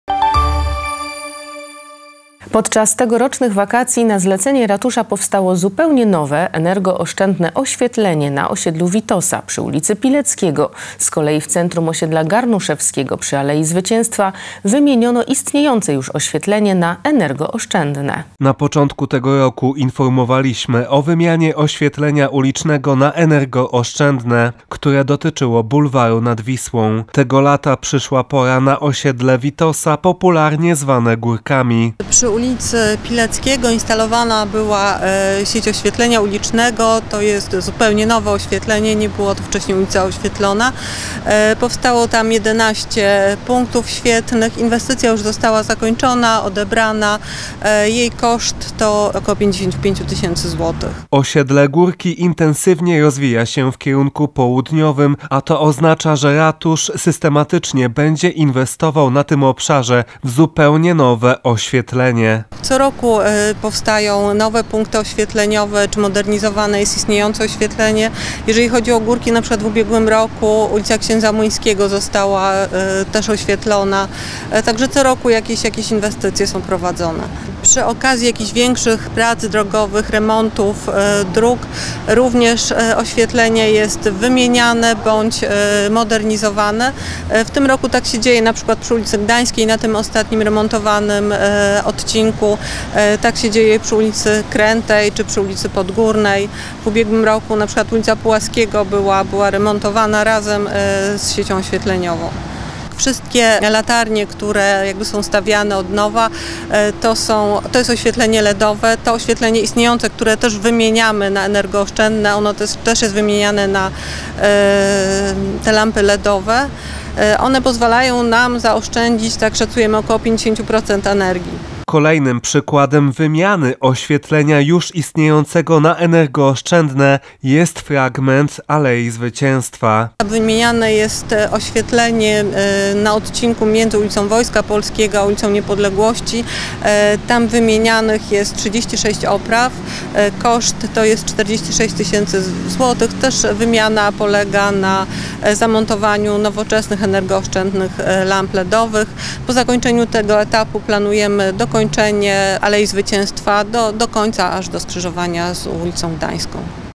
Ratusz inwestuje w oszczędne oświetlenie - Tv Tetka Tczew HD
Podczas tegorocznych wakacji na zlecenie ratusza powstało zupełnie nowe, energooszczędne oświetlenie na osiedlu Witosa przy ulicy Pileckiego. Z kolei w centrum osiedla Garnuszewskiego przy alei Zwycięstwa wymieniono istniejące już oświetlenie na energooszczędne.